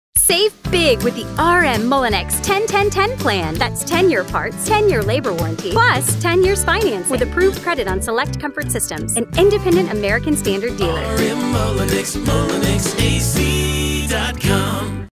RM Mullinix 10 Year Parts, 10 Year Labor, 10 Year Financing 0:15 Commercial
Right now, get 10 Years Parts + 10 Years Labor + 10 Years Financing on Qualifying American Standard Heating & Air Conditioning equipment! *with approved credit, call for details!